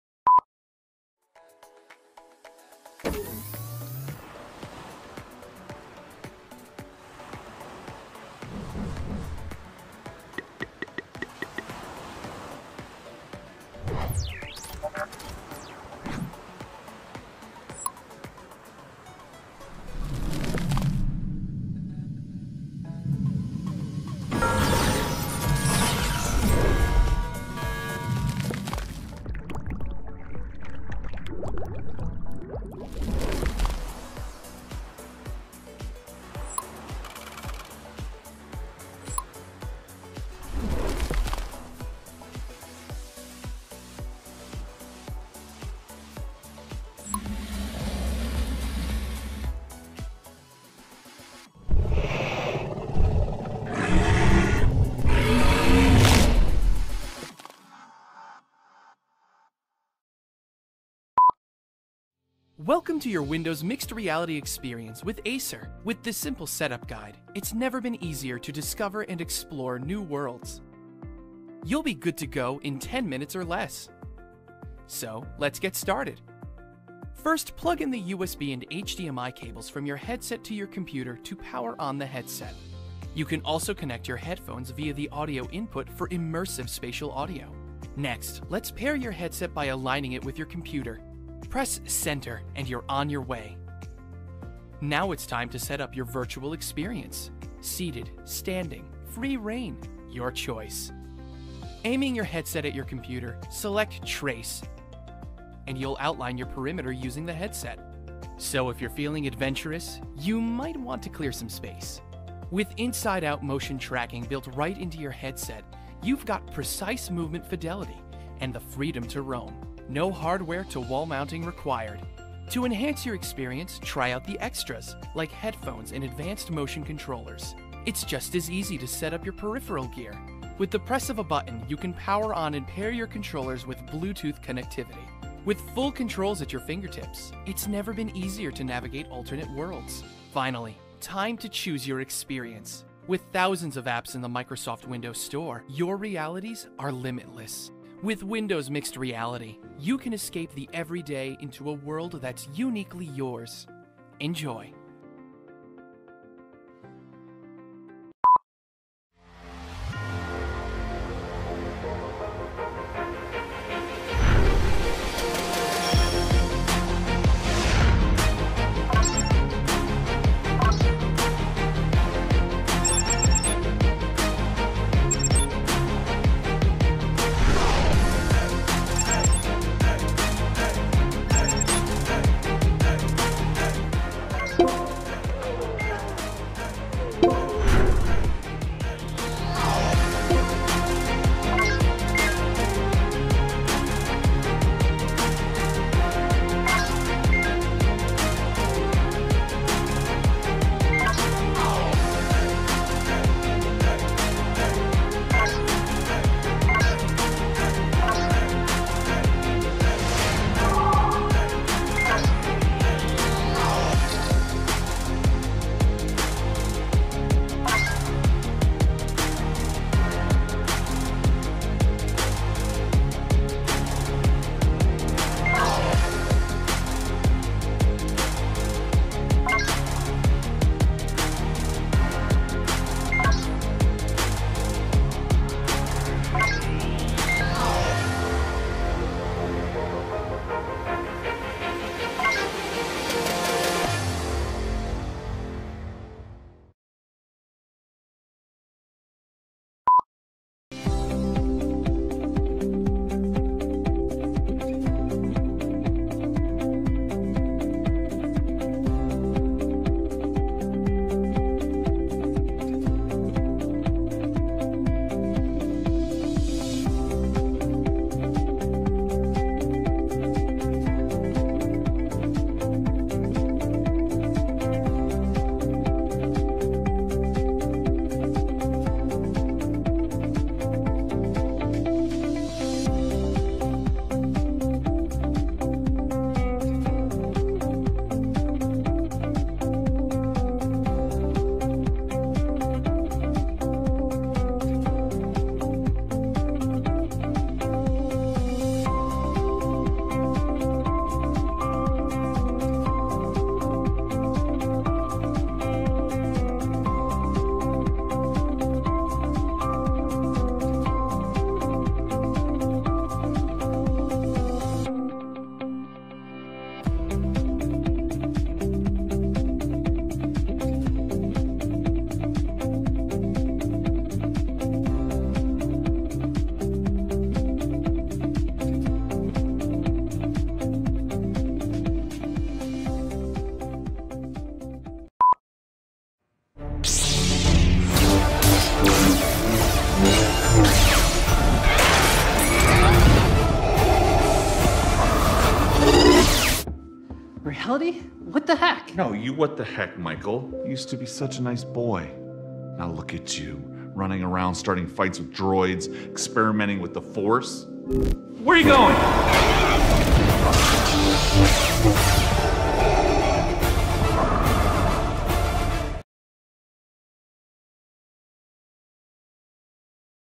Mixed Reality Headset Top 5 on Amazon
You Can Buy on Amazon

5- Lenovo G0A20001WW Explorer Mixed Reality Headset

4- Acer (AH101-D8EY) Windows Mixed Reality Headset Model VD.R05AP.002

3- Samsung Hmd Odyssey Windows Mixed Reality Headset with 2 Wireless Controllers (XE800ZAA-HC1US)

2- VR Headset, Pimax 5K Plus 120Hz Refresh Rate Virtual Reality Headset with Wide 200°FOV, Dual 2560x1440p RGB LCD Panels & 6 DOF Tracking, 1-Year Warranty, [Headset Only]

1- Oculus Quest All-in-one VR Gaming Headset – 128GB